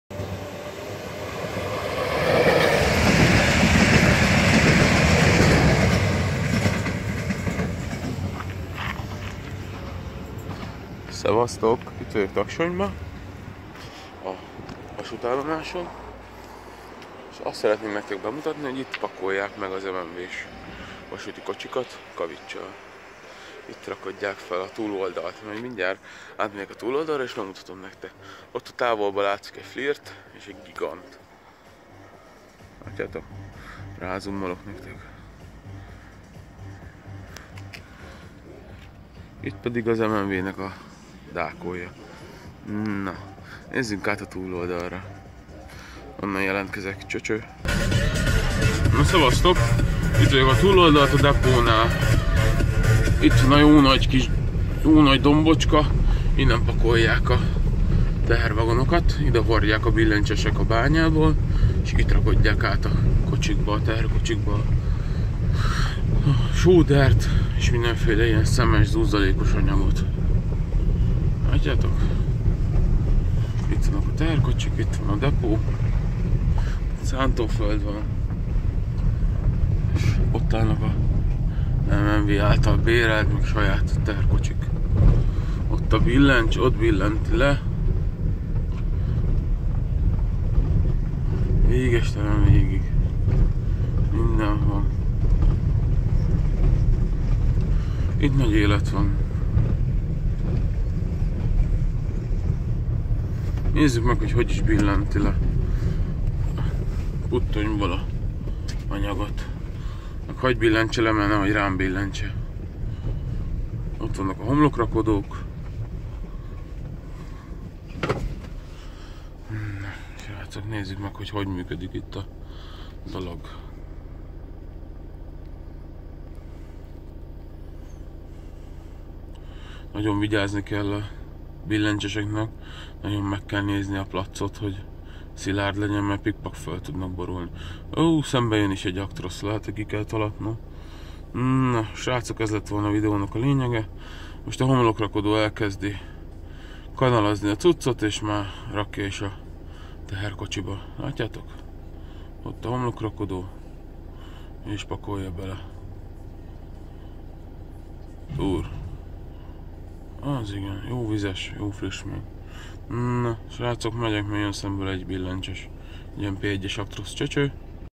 Taksonyi kavics depó
Taksony vasútállomás kavicsos vonatok felrakodása.